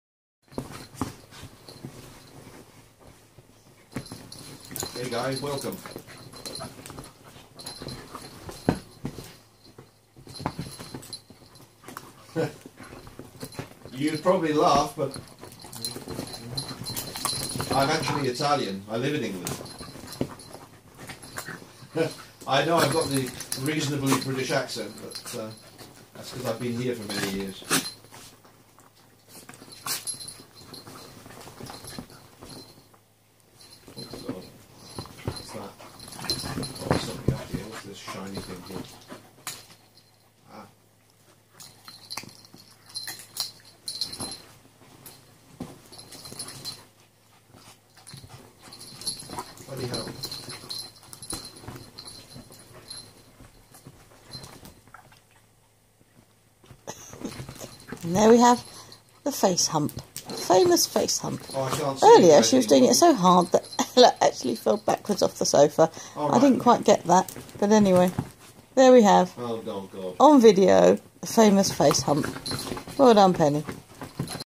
Ellabellabeagle : Dachshund Humps Her Face!
These two are best friends really, but Ella often pushes things too far.  No problem for Penny the Dachshund, she has a special way of putting her in her place!